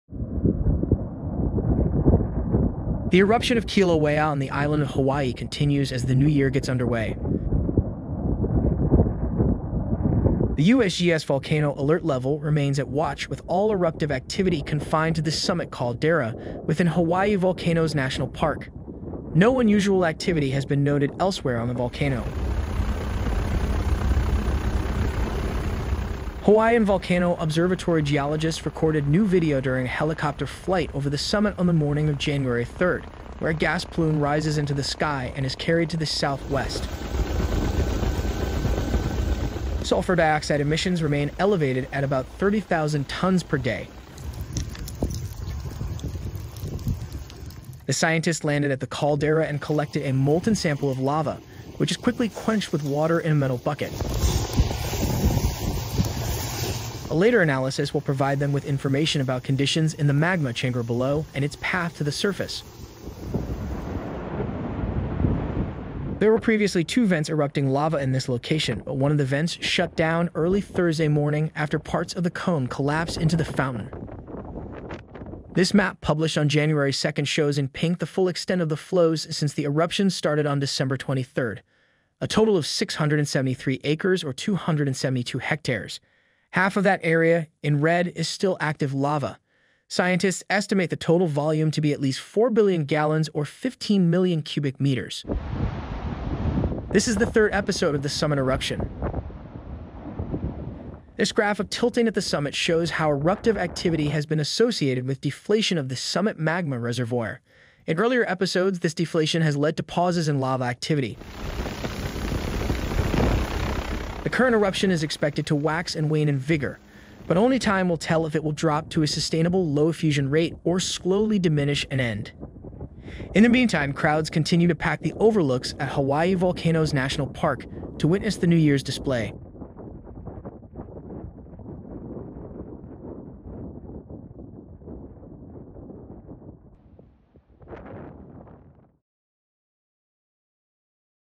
Kilauea Volcano Eruption Update for Friday - January 3, 2025
HAWAIʻI VOLCANOES NATIONAL PARK - The eruption of Kilauea volcano on the Island of Hawaiʻi continues into the year 2025.

A synthesized text-to-video voiceover was used in the narration for this story. All images and video are courtesy the U.S. Geological Survey and NPS.